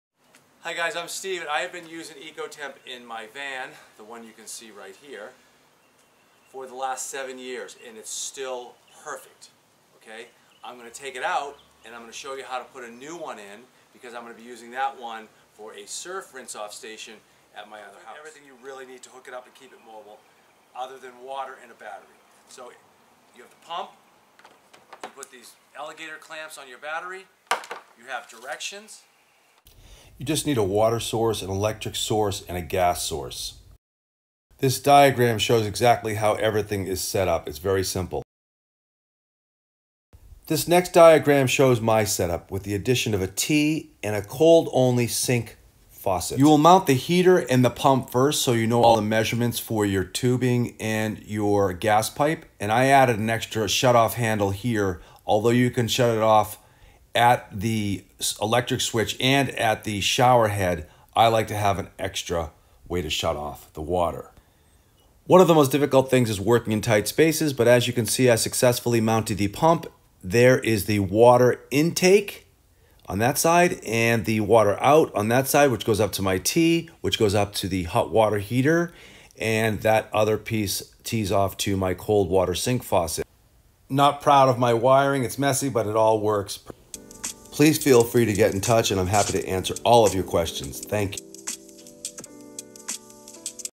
DIY Portable Tankless Water Heater Install for Van Conversion
Check out Steve's van conversion set up with the Eccotemp Luxe EL5 Portable Tankless Water Heater. Read his tips below!

Steve's Tips:
1. Mount the heater so that there is plenty of room for heat escape on the top of the unit! NOT close to combustibles.
2. For a Van install you’ll need: a 1/2” “T-joint”, 1/2 inch plastic tubing, “shark bite” connectors, pipe clamps, lever water shut off, wire caps, water tank ( I suggest a rectangle 15 gallons), electric shut off switch, wire caps, cold water fixture, 12V battery for pump, propane (only) 20lb tank, a plastic water fill and vent plate, and patience.
3. Tools: Utility knife, Phillips screw driver, (maybe a rotary tool for trimming), and a cordless drill.
4. Shower OUTSIDE of the van. Make a suction cut shower head mount you can place and remove after use.
5. A grey water tank is NOT necessary if you are not dumping pollutants from your sink. Simply drain under your van.